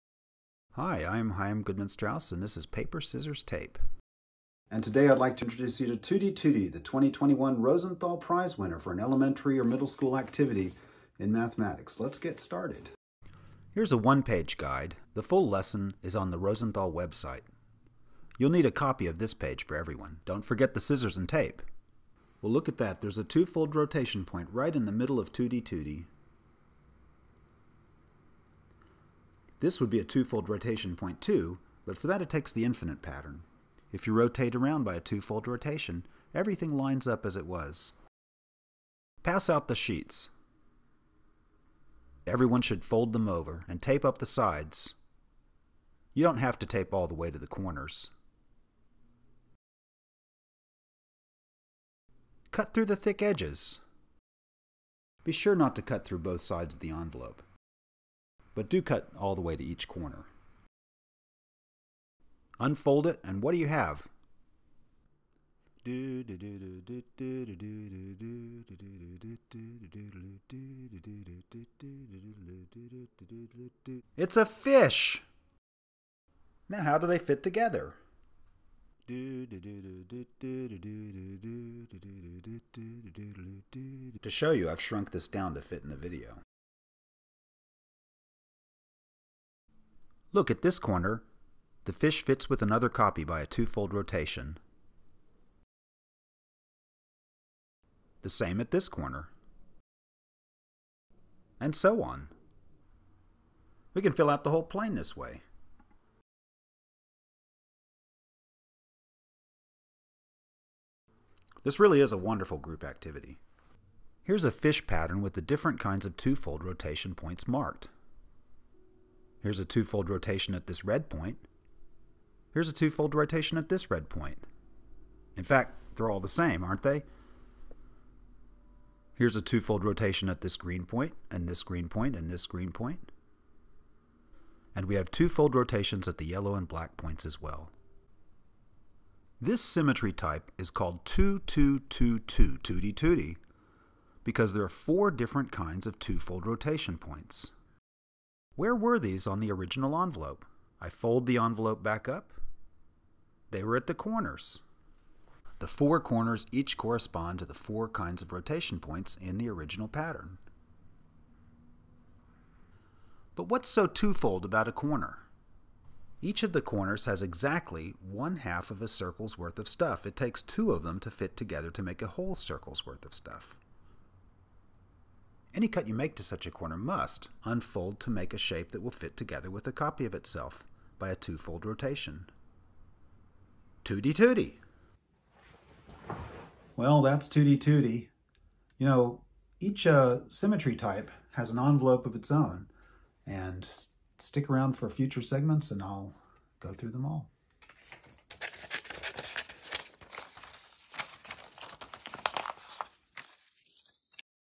Tooti Tooti!
Paper Scissors Tape presents:  Tooti Tooti, the winner of the 2021 Rosenthal Prize for Inspiration and Innovation in Math Teaching, bringing John Conway's orbifolds to the classroom!

Don't just watch the video: get some kids together, download the files and try it out!